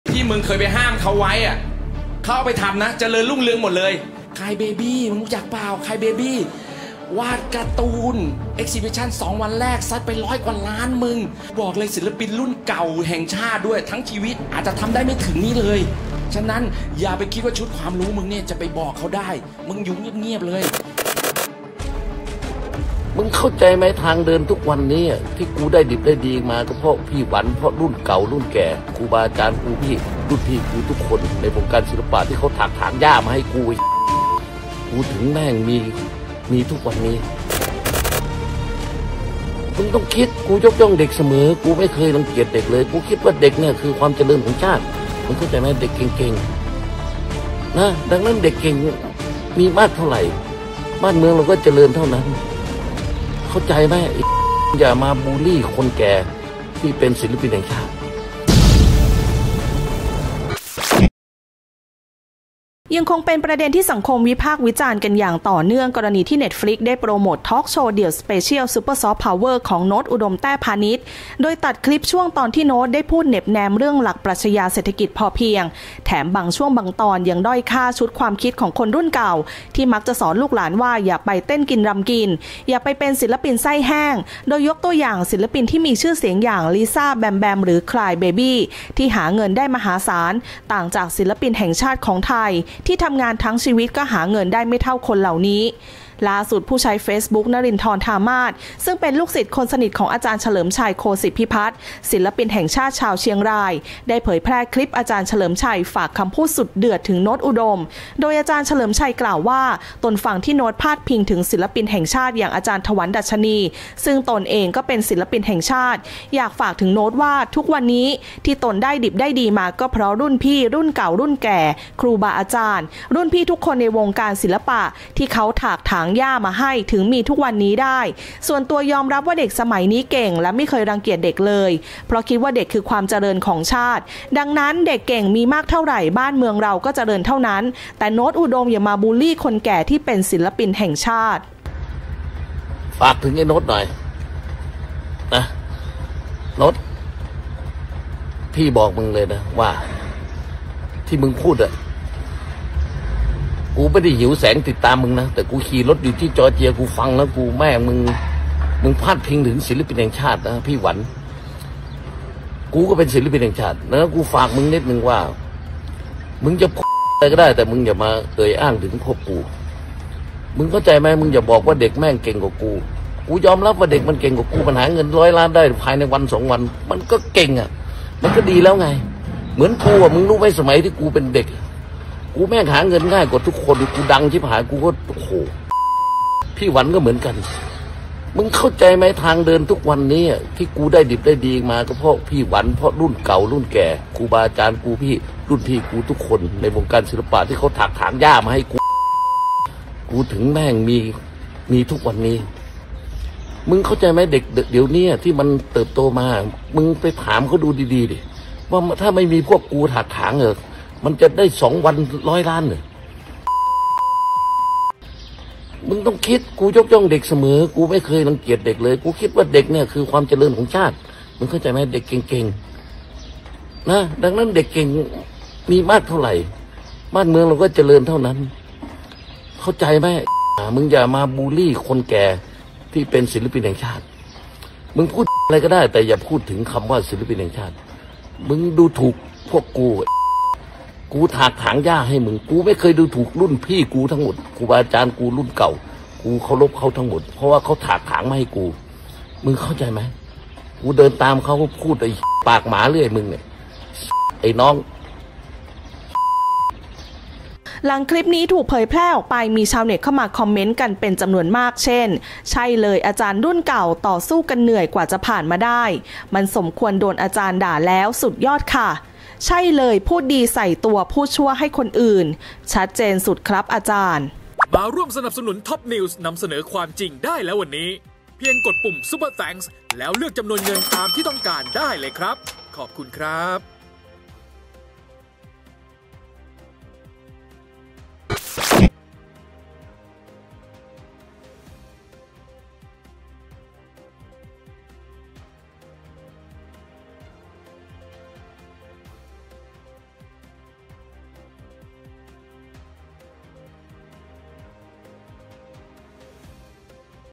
เดือด! “อ.เฉลิมชัย” จอดรถด่า “โน้ส” ข้ามประเทศ ปากสุนัขแขวะ “อ.ถวัลย์” | TOPUPDATE
เดือดจัด! “อ.เฉลิมชัย” ฉุนขาด ถึงขั้นจอดรถด่า “โน้ส อุดม” ข้ามประเทศ ลั่นปากสุนัข-บูลลี่คนแก่ ปมพาดพิง “อ.ถวัลย์” ก่อนสั่งสอนชุดใหญ่ หากไม่มีคนแก่ถากถางหญ้ามาให้ เด็กมันจะได้ดีเหมือนวันนี้ไหม